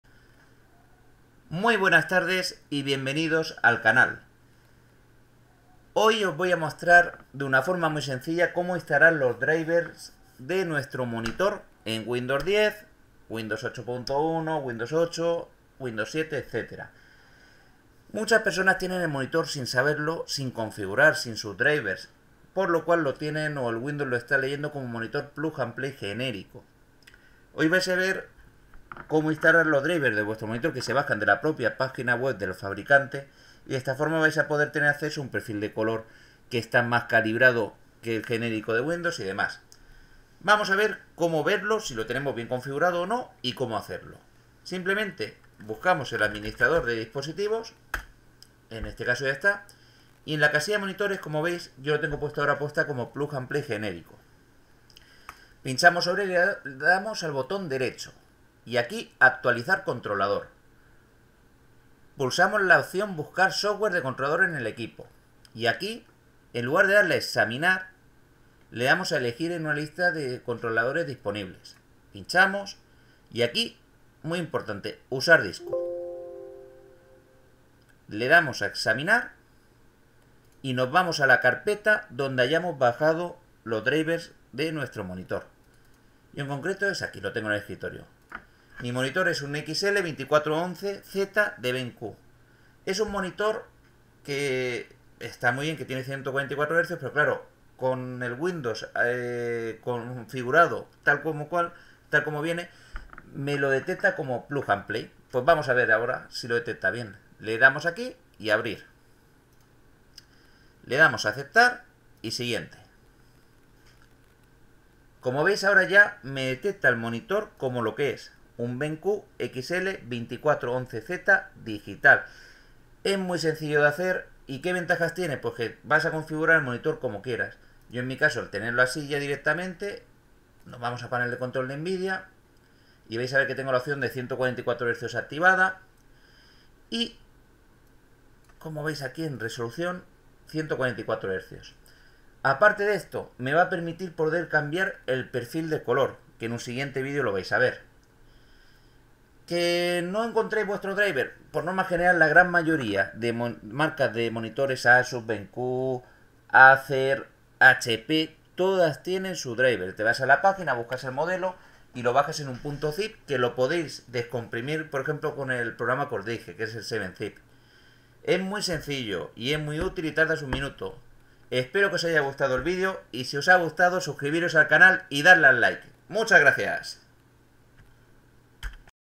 Instalar Driver de monitor 2024
Un breve video donde explico de una forma muy sencilla como instalar los drivers de nuestro monitor. Valido para Windows 10, Windows 8, Windows 7... Funciona en Nvidia y amd.